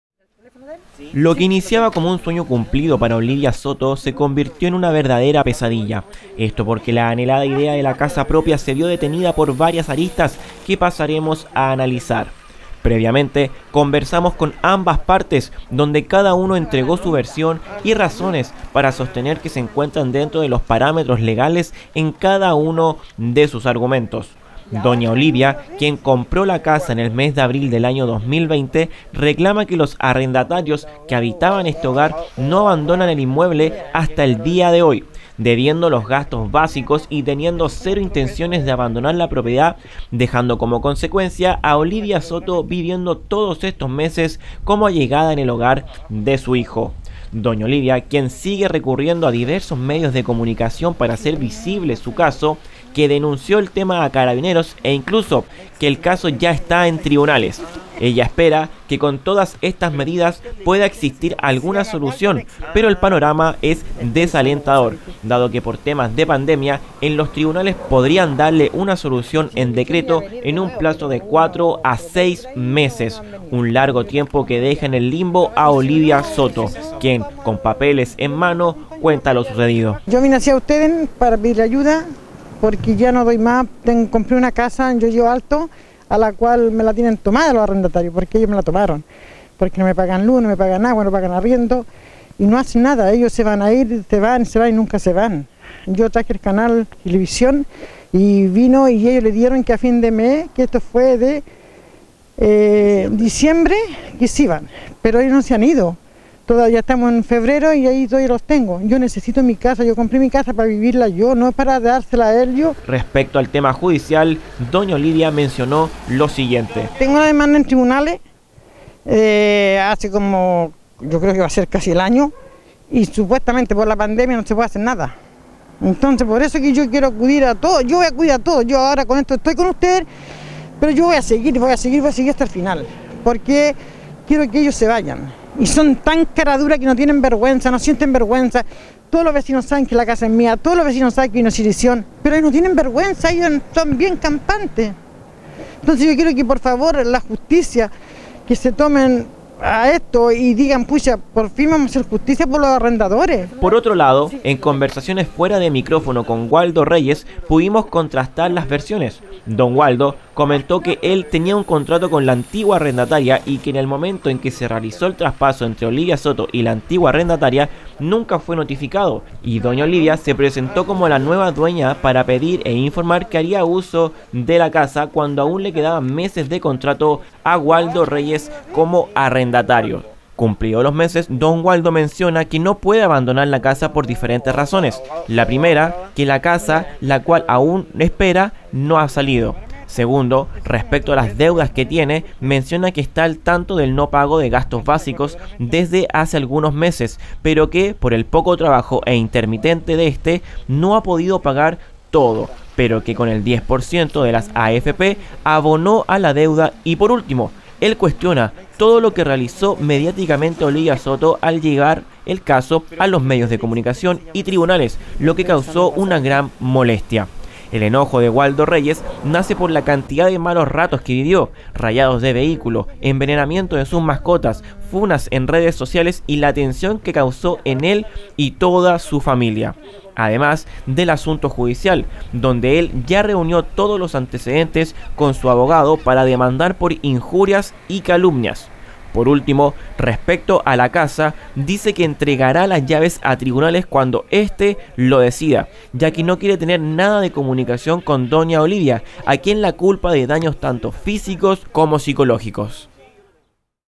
08 FEB 2021 DRAMA POR COMPRA DE CASA ARRENDADA
OSCUROS MESES HAN PASADO OLIVIA SOTO Y WALDO REYES, QUIENES SE ENCUENTRAN ACTUALMENTE EN UN CONFLICTO POR UNA CASA QUE COMPRÓ DOÑA OLIVIA, LA CUAL AÚN NO HABITA PORQUE LA FAMILIA DE DON WALDO, QUIEN ARRENDABA EL INMUEBLE HACE MESES A LA ANTIGUA DUEÑA Y QUE TENÍA UN CONTRATO VIGENTE SE REHÚSA A DEJAR, ARGUMENTANDO QUE TIENEN LOS PAPELES EN REGLA Y EL TIEMPO ESTIPULADO PARA DEJAR LA CASA DE MANERA PAULATINA.